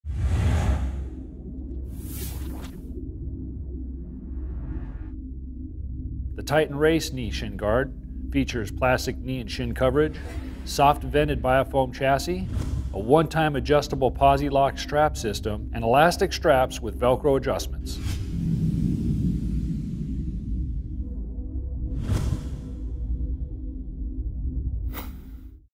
2010 Fox Racing Titan Race Knee/Shin Guards
Full plastic knee and shin coverage, soft vented bio-foam chassis, adjustable posi-lock strap system.